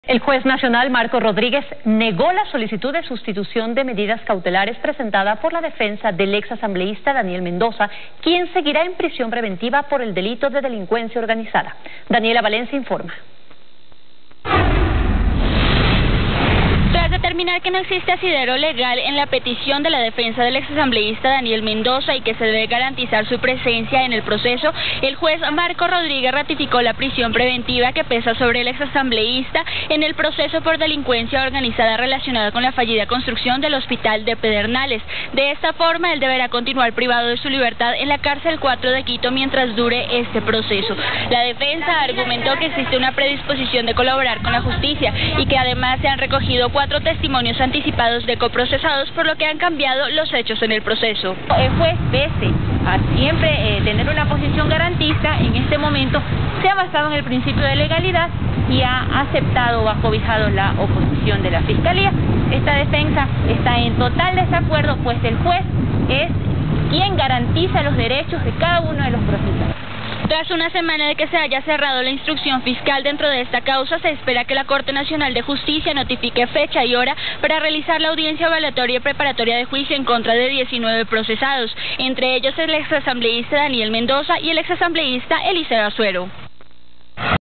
RATIFICAN PRISIÓN PREVENTIVA CONTRA DANIEL MENDOZA, 13/10/2020, TELEAMAZONAS
RATIFICAN PRISIÓN PREVENTIVA CONTRA DANIEL MENDOZA
*NOTA DE TELEAMAZONAS